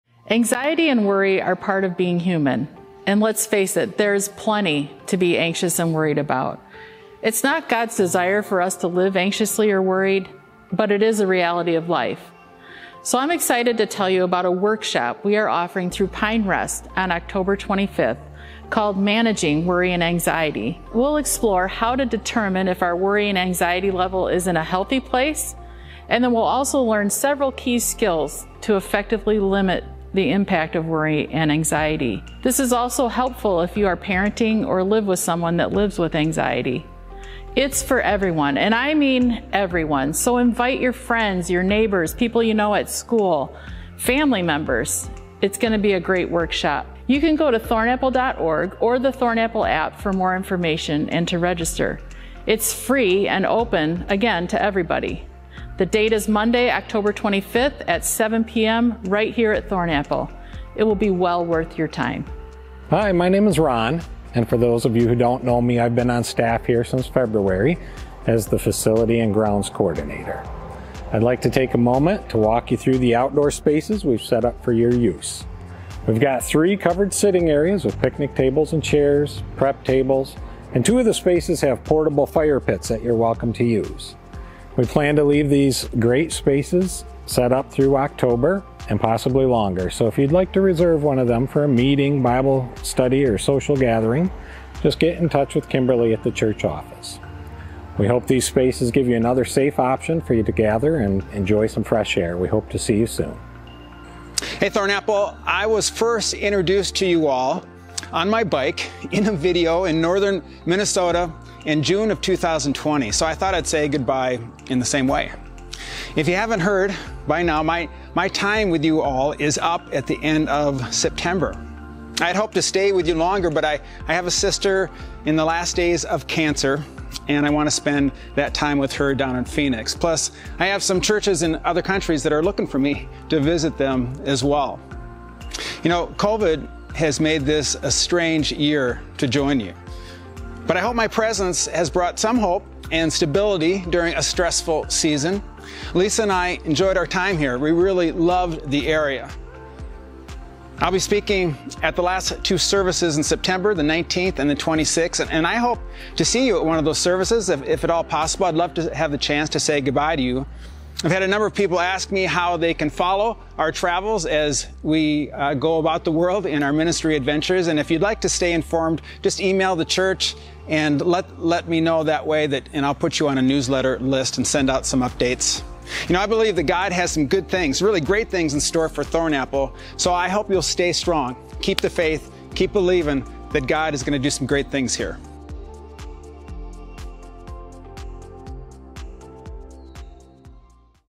September 2021 | 3 Things at Thornapple
1. Worry & Anxiety Workshop
2. Outdoor spaces for you to use at Thornapple
3. Parting thoughts from Pastor Remy